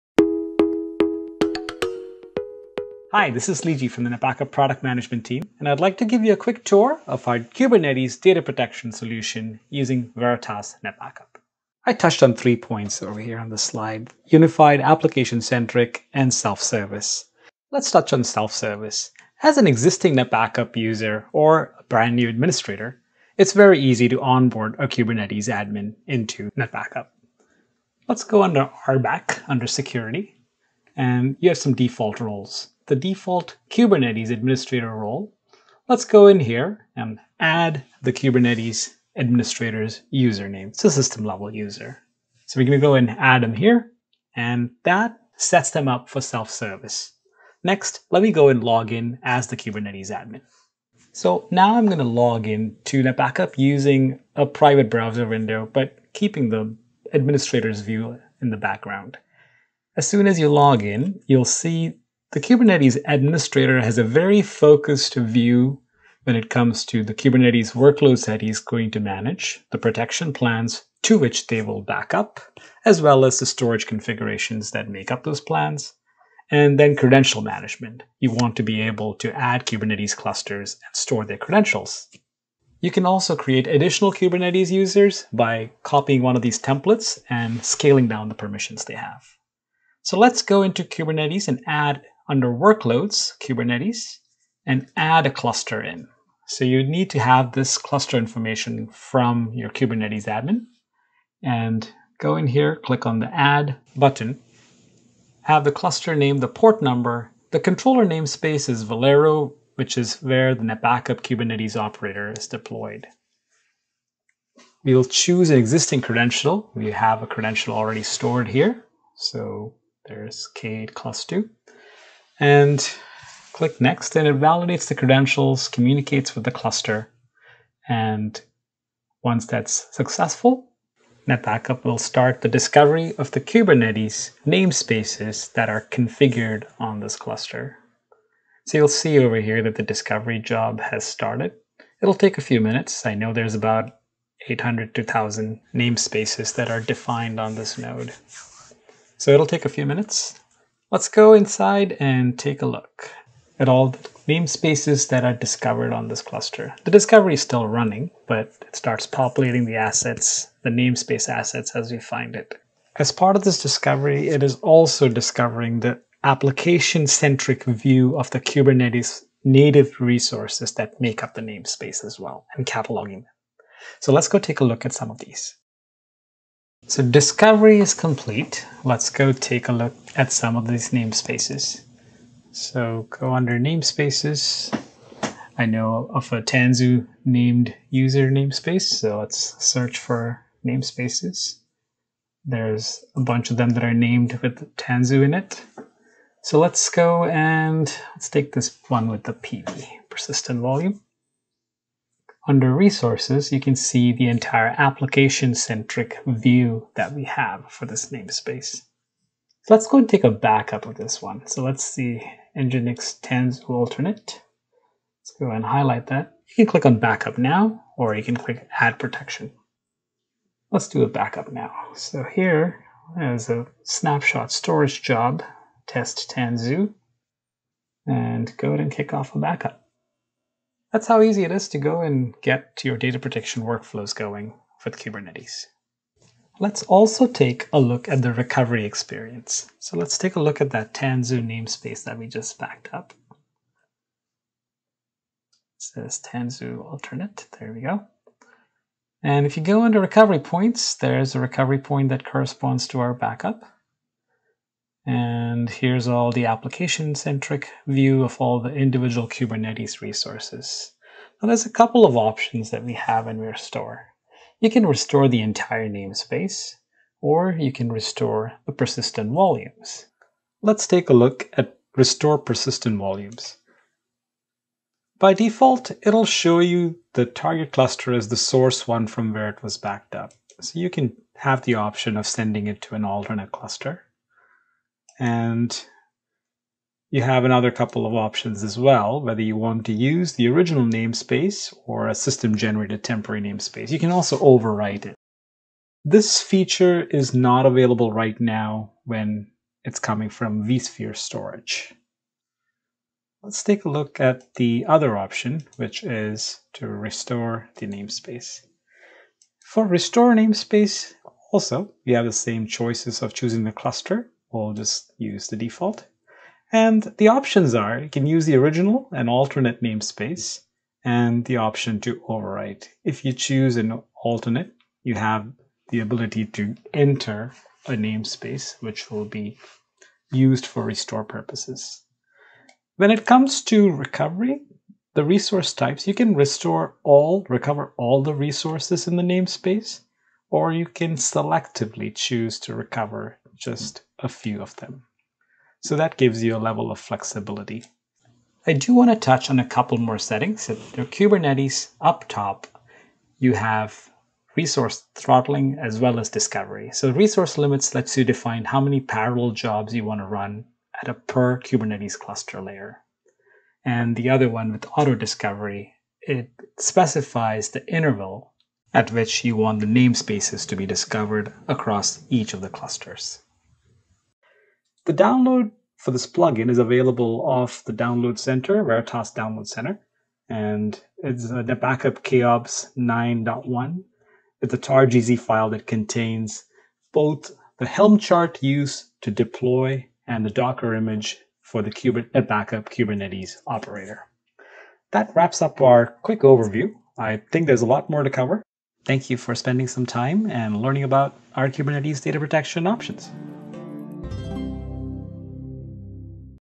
Data Protection for Kubernetes with NetBackup
Configure data protection for all of your Kubernetes clusters and namespaces with NetBackup in a few simple mouse clicks.